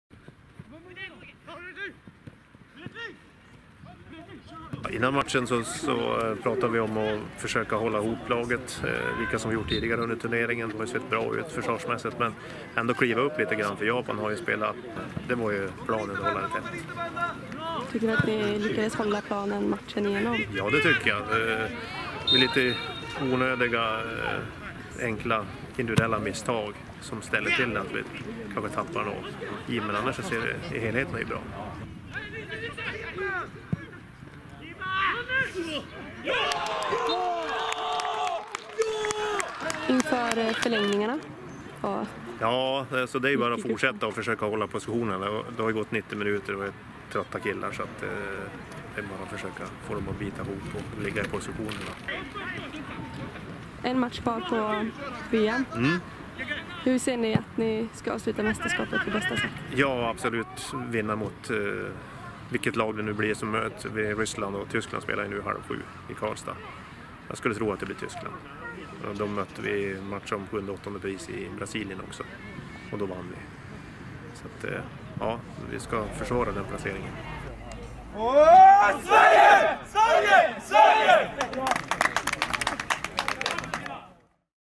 Match om sjunde- åttondeplats: "Vi ska försvara den placeringen"
Sverige - Japan går till förlängning och straffavgörande där Japan vinner matchen.
Spel om sjunde- åttondeplats i INAS-VM för Sverige den 17:e augusti.